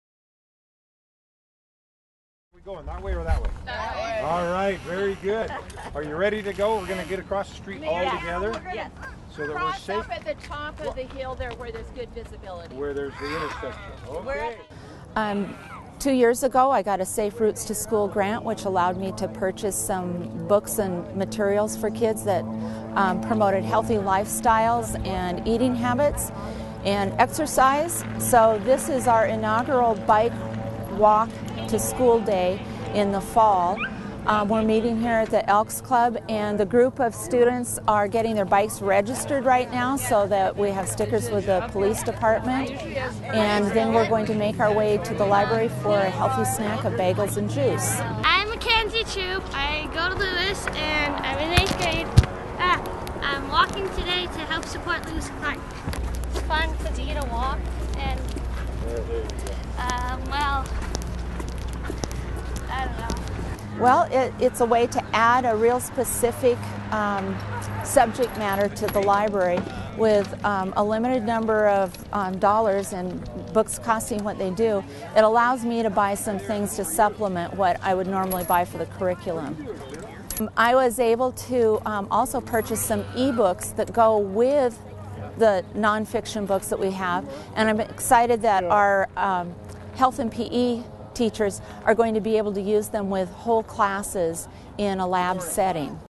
Billings students take part in International Walk to School Day
Lewis and Clark Middle School students, faculty and parents participated in International Walk to School Day in order to encourage more active and healthy lifestyles.